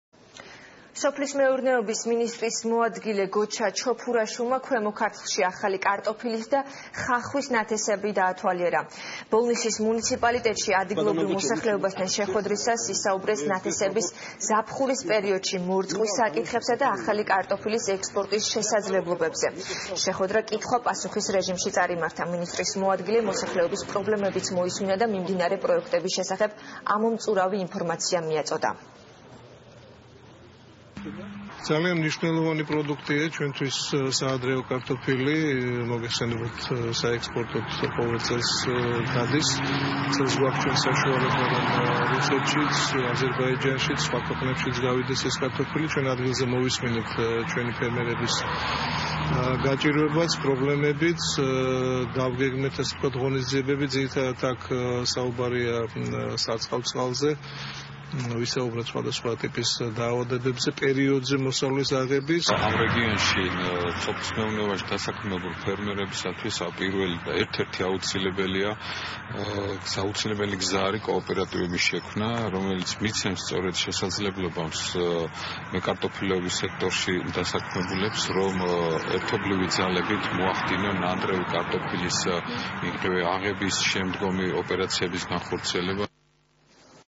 შეხვედრა ბოლნისში
ბოლნისის მუნიციპალიტეტში ადგილობრივ მოსახლეობასთან შეხვედრისას საუბარი შეეხო ნათესების ზაფხულის პერიოდში მორწყვის საკითხებს, ახალი კარტოფილის ექსპორტის შესაძლებლობებს. შეხვედრა კითხვა-პასუხის რეჟიმში წარიმართა. მინისტრის მოადგილემ მოსახლეობის პრობლემებიც მოისმინა და მიმდინარე პროექტების შესახებ ამომწურავი ინფორმაცია მიაწოდა.